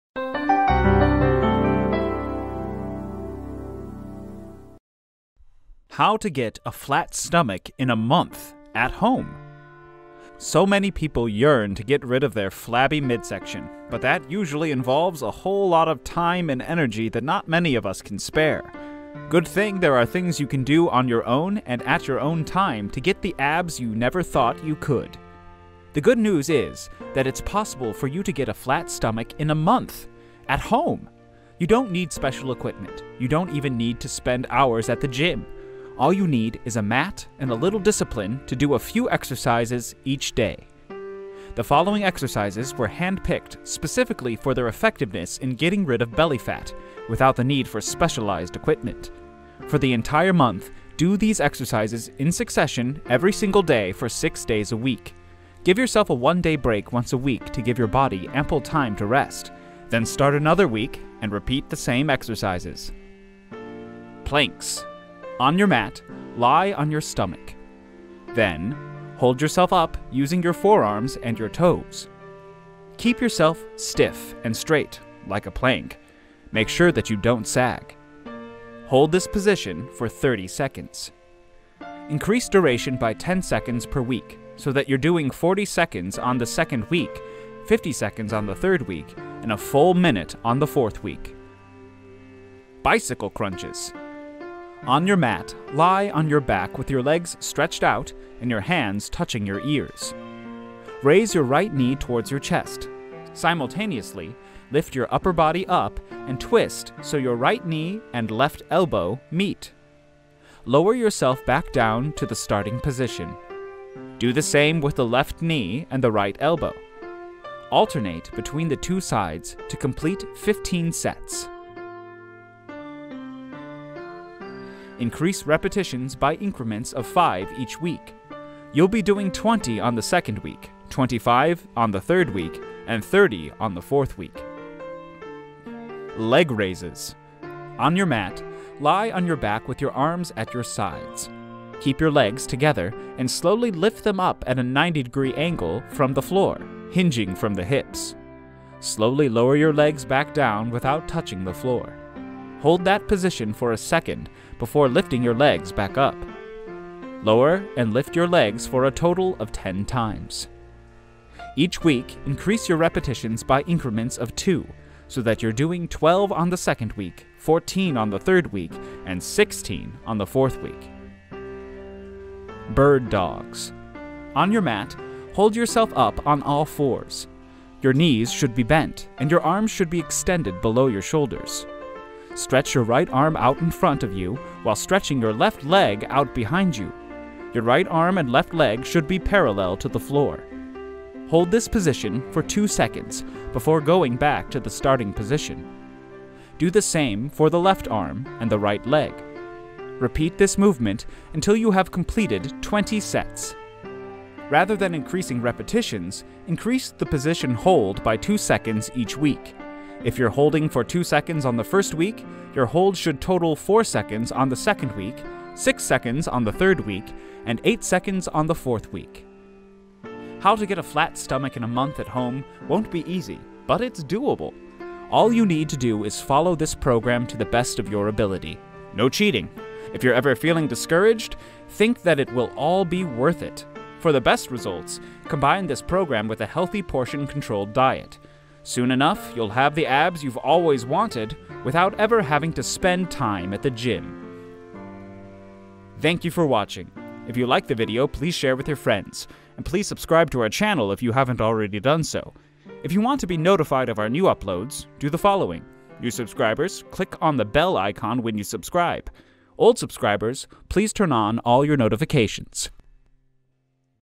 How to Get a Flat Stomach in a Month at Home - Abs Workout Planking
This is how to get a flat stomach in a month at home. If you want to get rid of your belly fat, these few belly fat burning exercise should help you do just that right from your home. Watch the video now to learn how simple it is to get a flat stomach in a month from home.
Learn more about plank, and plank workouts, flat stomach 30-day challenge, and much more. Waysandhow.